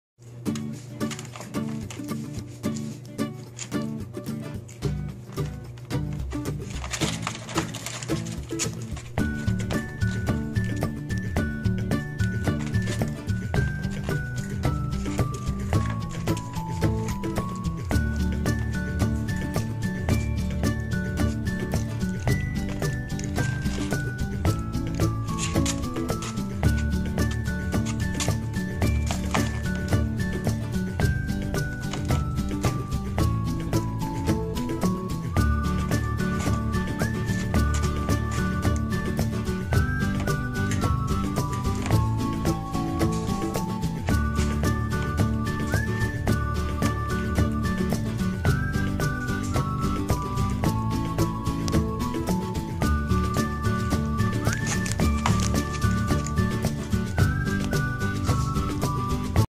Cardigan welsh corgi puppies
Cc &Hershey having fun learning how to eat solid food